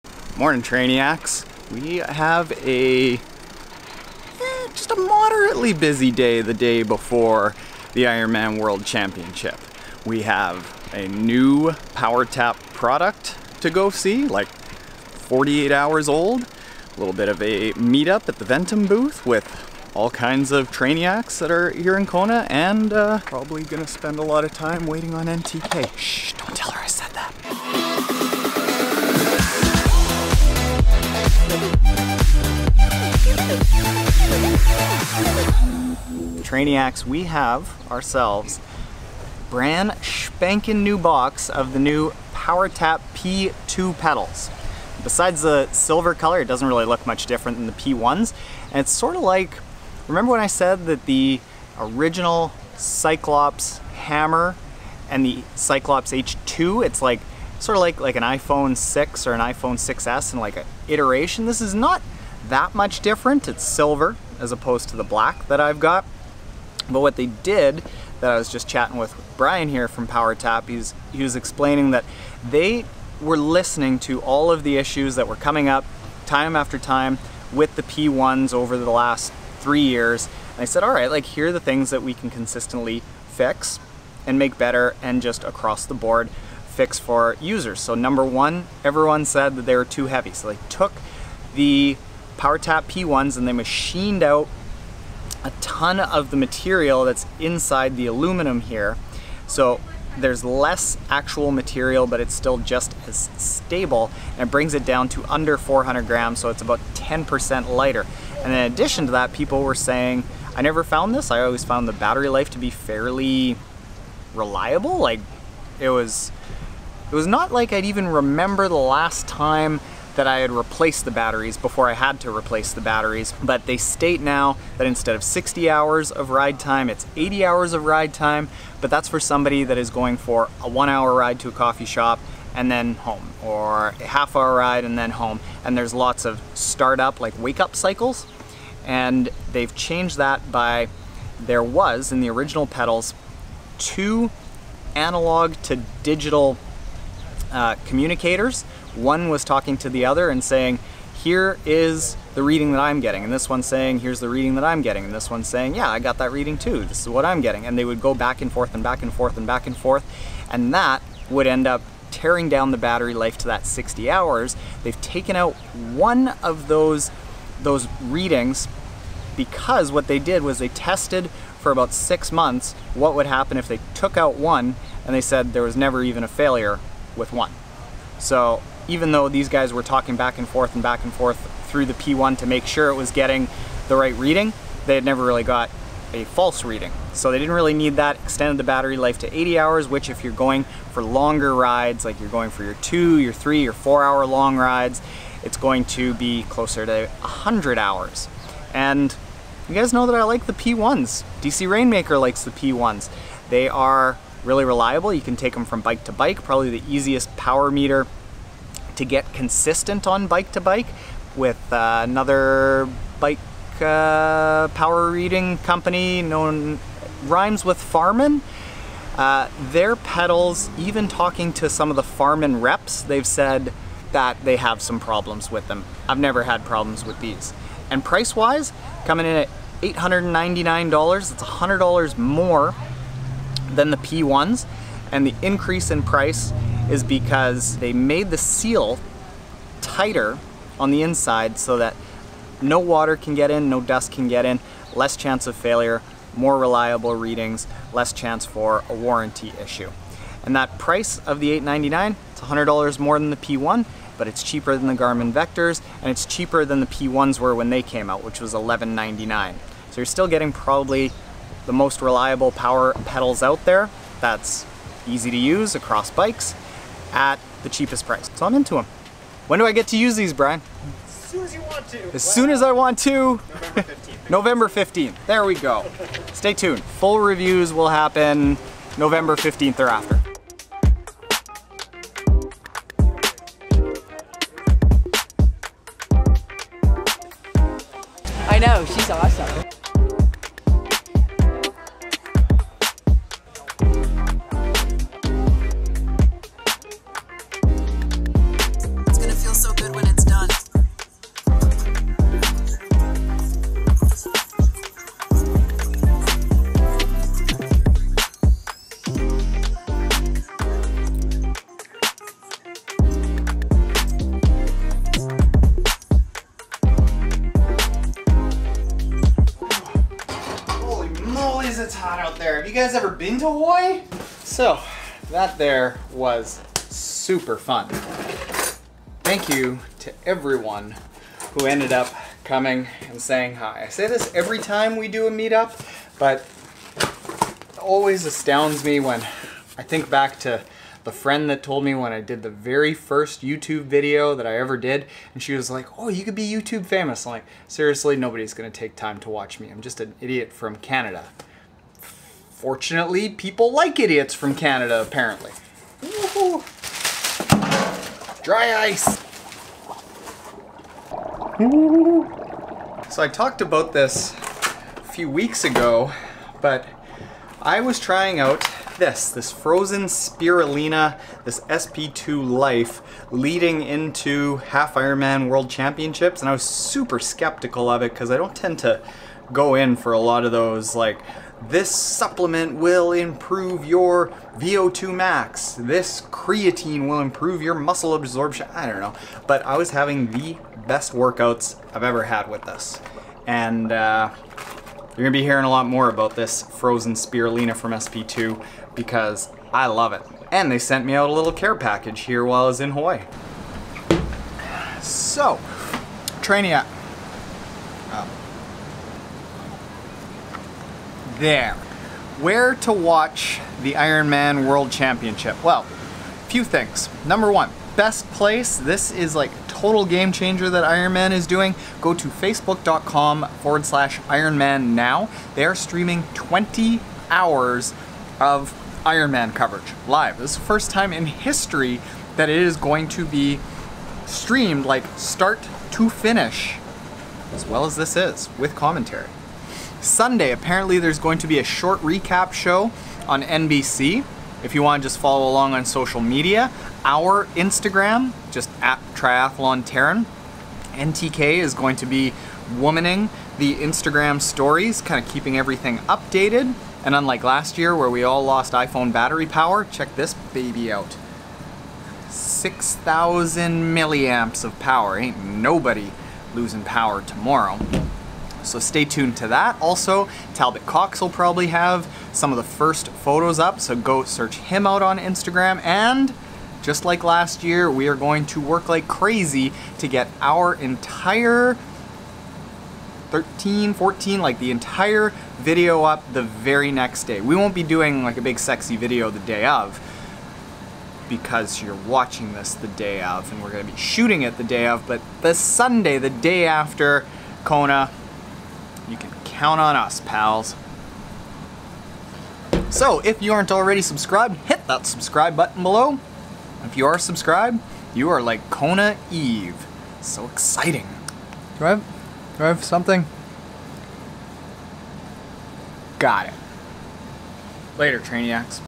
IRONMAN HAWAII WORLD CHAMPIONSHIP: PowerTap P2 Power Pedals
Triathlon Taren shows of the brand new PowerTap P2 Power Pedals, meets Trainiacs at the Ventum booth and lists the different places that viewers can keep up with the Ironman Hawaii World Championship.